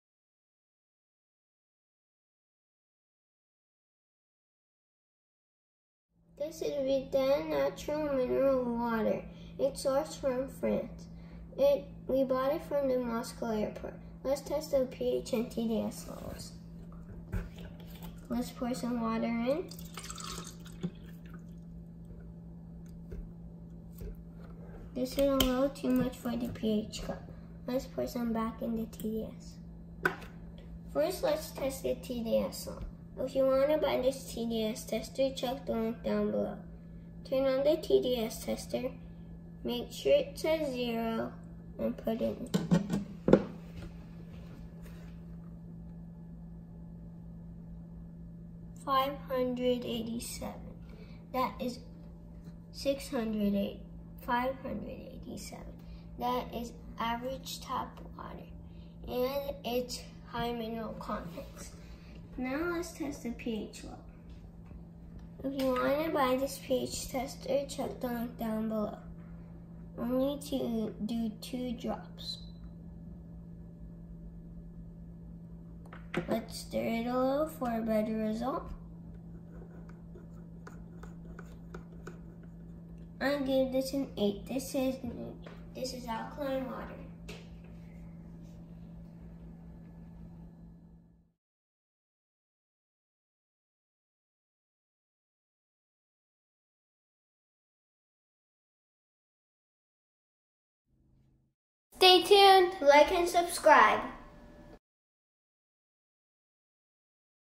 Vittel Mineral Water test - pH and TDS
Mineral Water from Vittel Grand Source, France. A brand of Nestlé Waters

Results:
pH: 8
TDS: 587